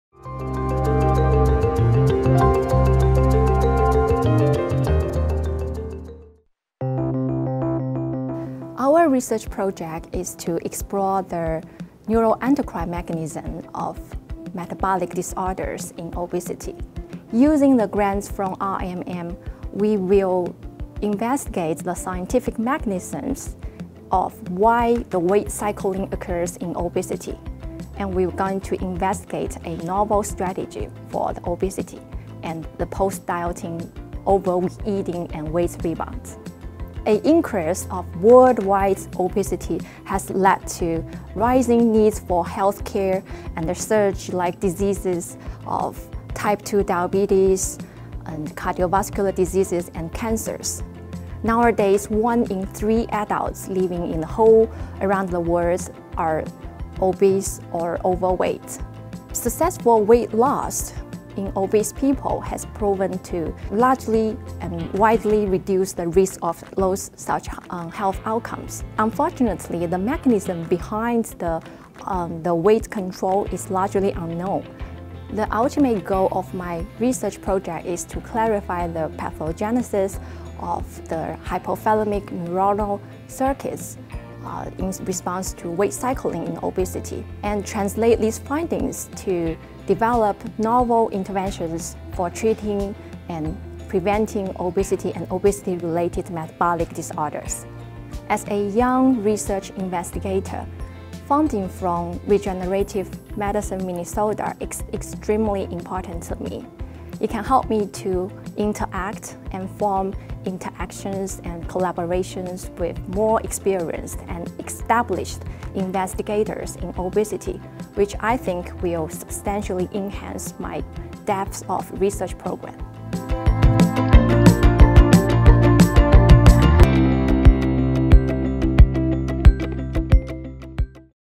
Ping Chen Interview
Dr. Ping Chen, PhD, is a research grant recipient from Regenerative Medicine Minnesota. The major challenge in treating obesity is not just losing weight, but preventing weight from coming back. This proposal examines a new treatment strategy of using a gene transfer therapy to safely target brain mechanisms that control hunger and energy output. The investigator’s previous research indicates that gene transfer of a well-tolerated liver enzyme could revolutionize clinical treatment of obesity, making it easier for obese patients to lose weight safely and minimize later weight gain.